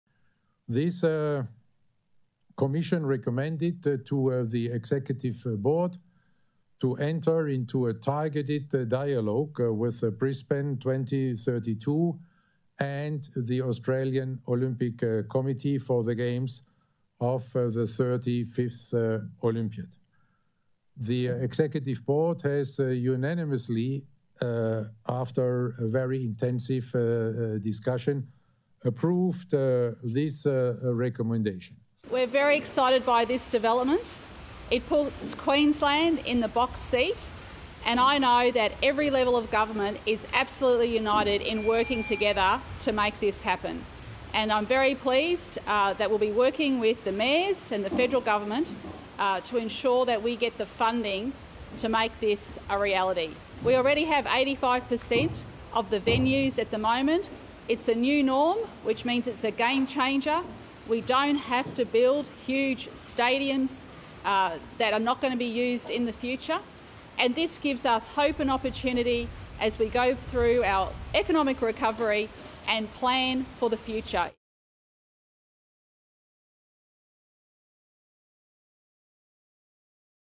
IOC backs Brisbane as 'preferred partner' to host 2032 Olympic Games
Brisbane is in the box seat to host the 2032 Olympic and Paralympic Games after it was named the International Olympic Committee's "preferred partner" for the event. If the Queensland city wins the bid, it will be Australia's third time hosting the Olympics. IOC president, Thomas Bach, said on Wednesday the IOC's executive board had unanimously approved the recommendation of the bid, which has the support of the federal and state governments
Brisbane chosen as 'preferred partner' to host 2032 Olympic Games, IOC says